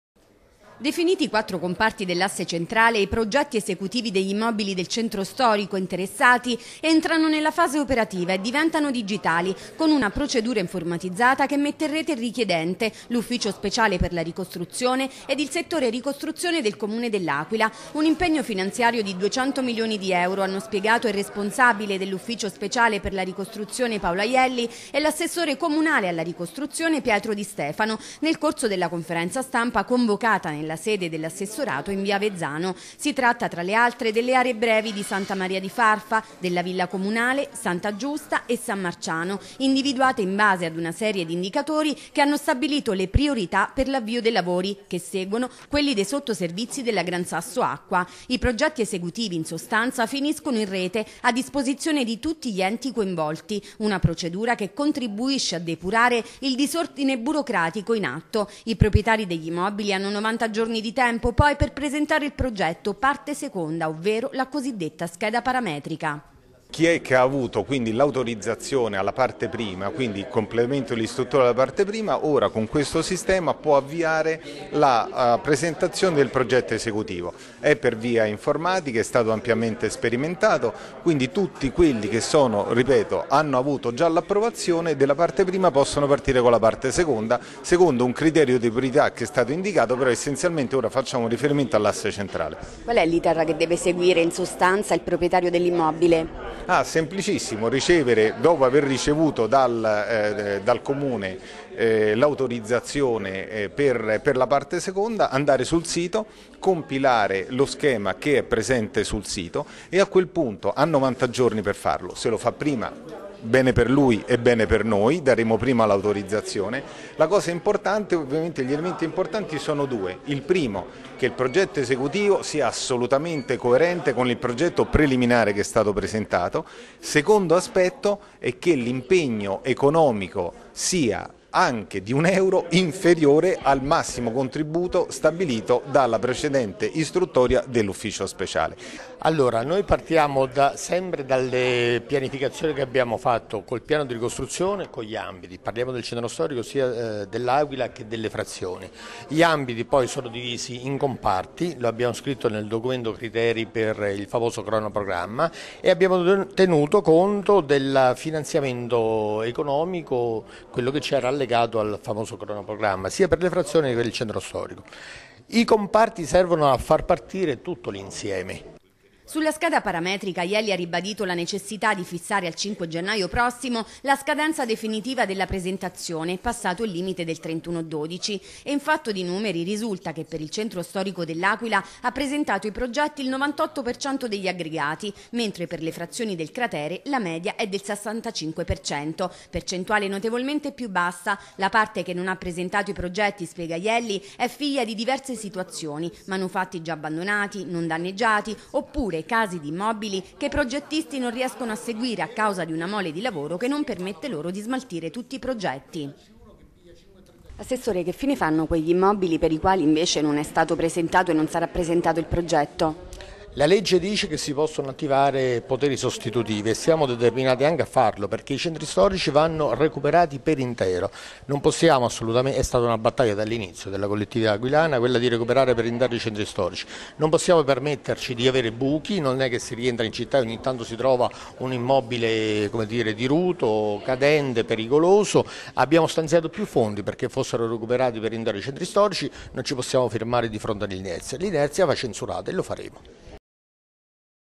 ASSE CENTRALE: PROCEDURA INFORMATIZZATA PER PROGETTI ESECUTIVI
Definiti i quattro comparti dell'Asse Centrale, i progetti esecutivi degli immobili del centro storico interessati entrano nella fase operativa e diventano digitali con una procedura informatizzata che mette in rete il richiedente, l'Ufficio Speciale per la Ricostruzione ed il settore ricostruzione del Comune dell'Aquila. Un impegno finanziario di 200 milioni di euro, hanno spiegato il responsabile dell'USRA Paolo Aielli e l'assessore comunale alla ricostruzione Pietro Di Stefano nel corso della conferenza stampa convocata nella sede dell'assessorato in via Avezzano.